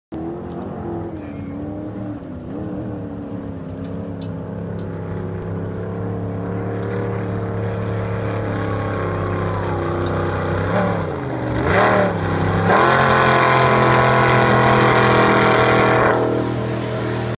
2014 Lake James jet boat fly by
2014 Classic Hot boats annual toys for tots meeting on Lake James